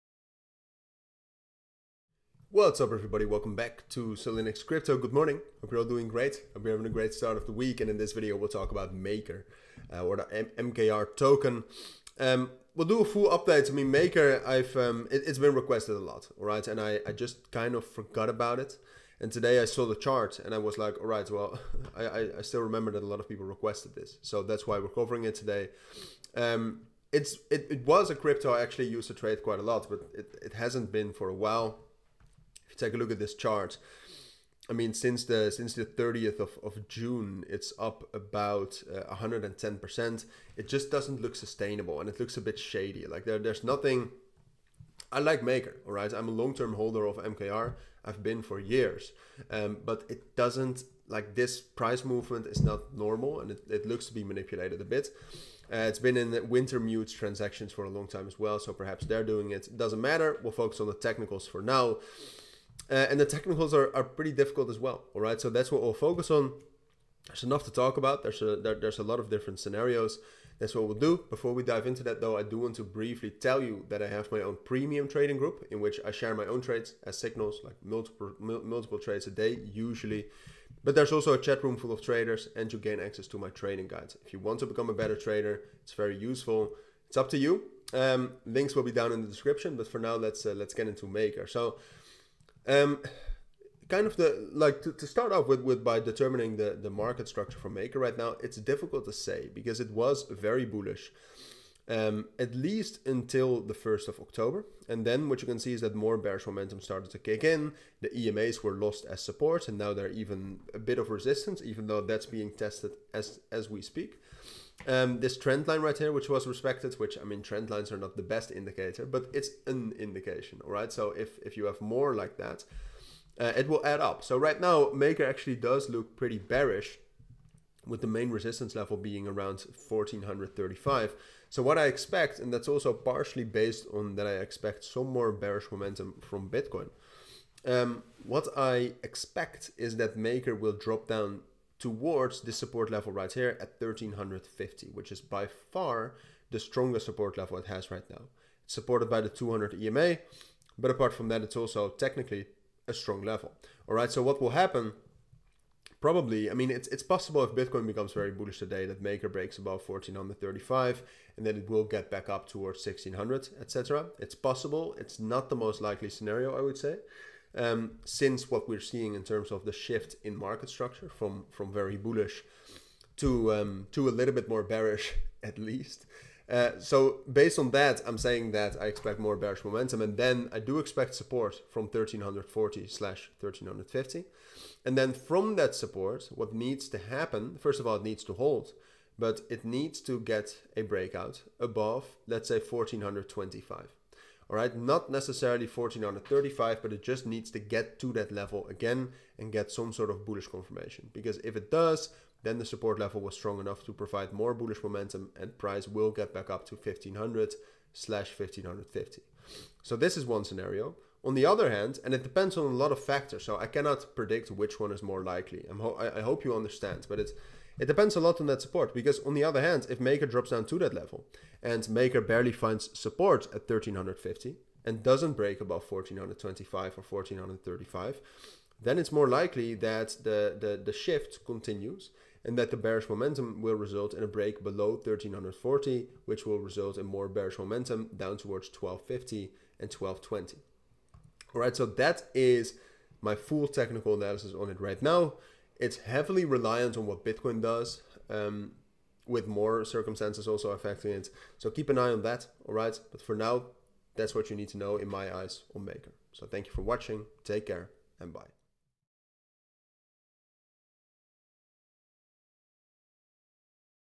WHY MKR MIGHT CRASH SOON!🔥 | MAKER (MKR) PRICE PREDICTION & NEWS 2023!
MAKER MKR PRICE PREDICTION 2023

Social links!